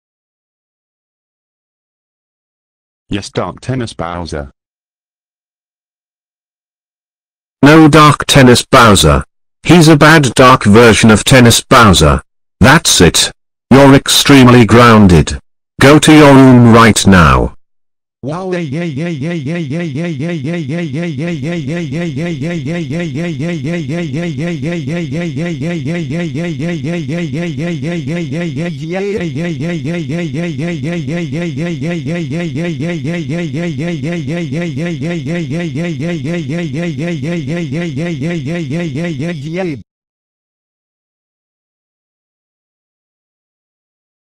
Dark Tennis Bowser Jr. Says Yes Dark Tennis Bowser/Grounded
Dark Tennis Bowser Jr. says Yes Dark Tennis Bowser and he got grounded by Black King Bowser.